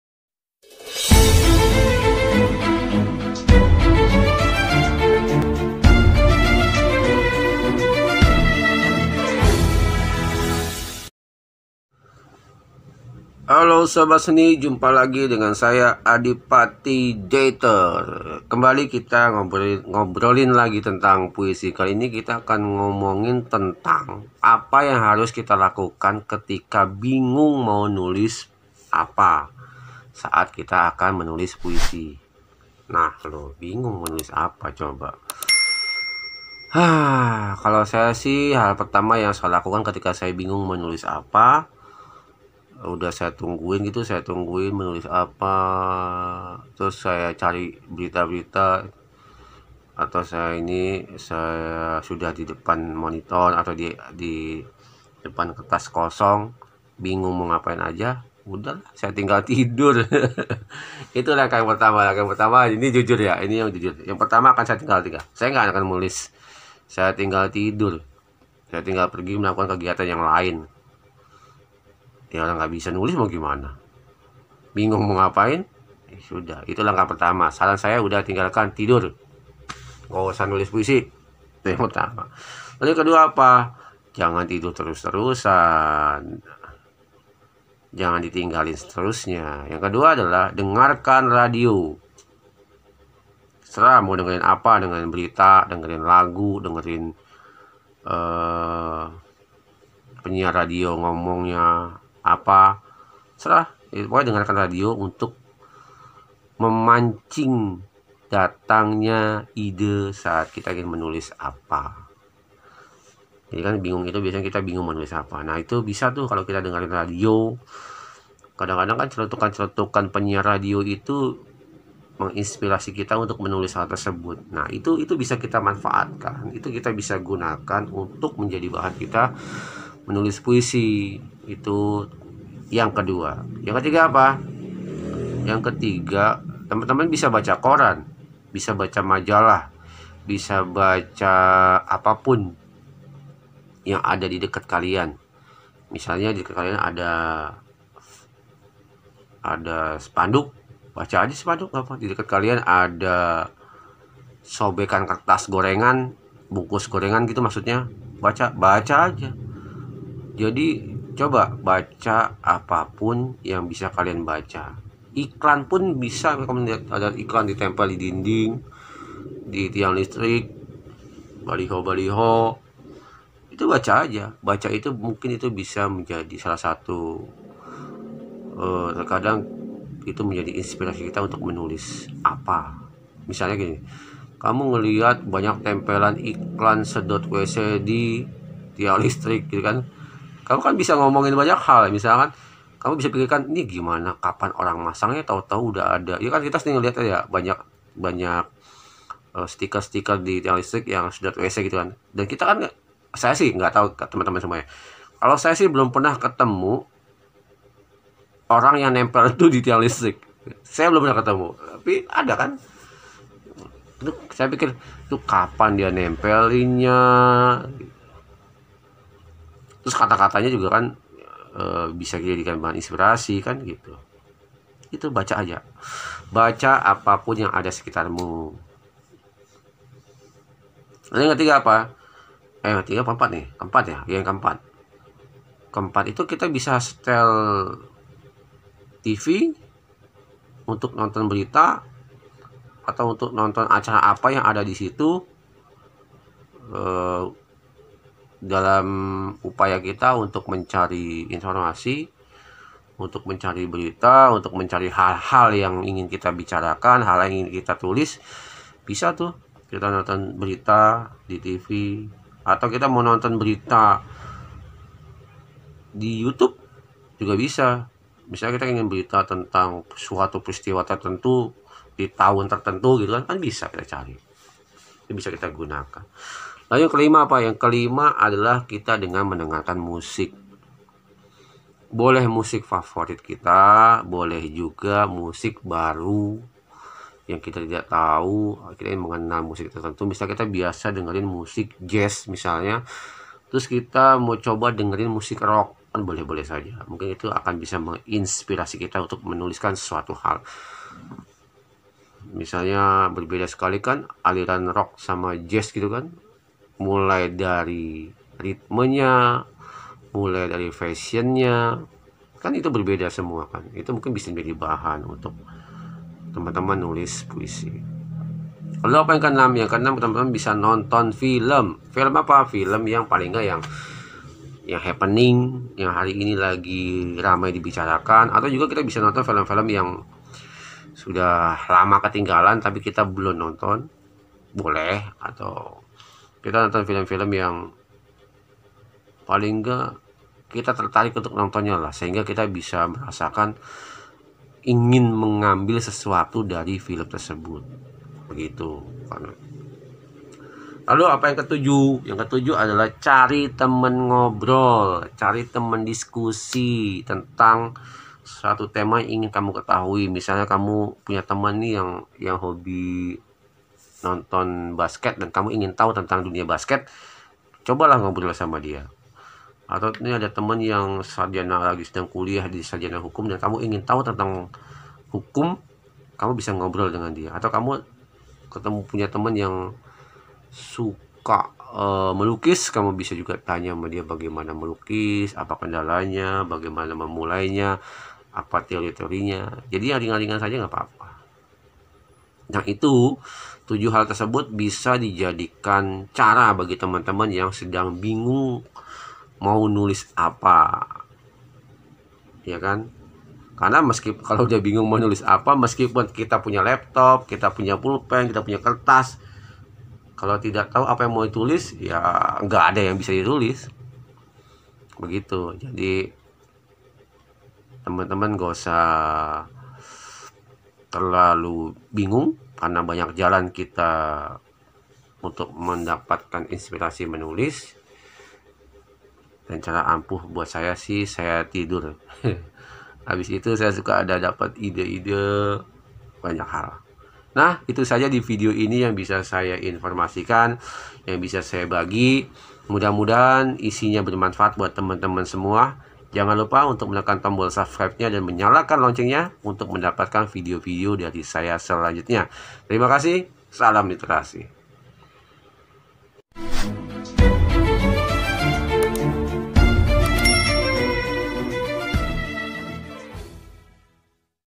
KETIKA PENYAIR BINGUNG MAU NULIS APA
Video ini berisi tentang apa yang harus dilakukan ketika seorang penyair bingung mau nulis apa.

yuk simak bareng-bareng.